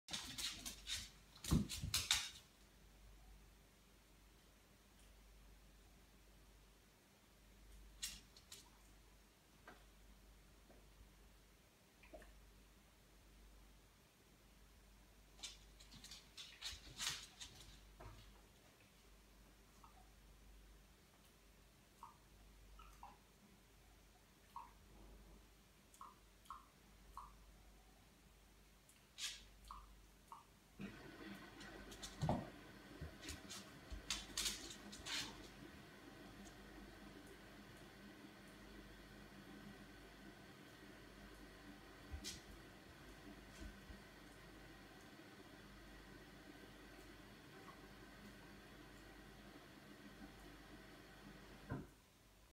Staffordshire bullterrier playing with British shorthair cat
Sigrid and Winston is playing, friends forever ❤️💙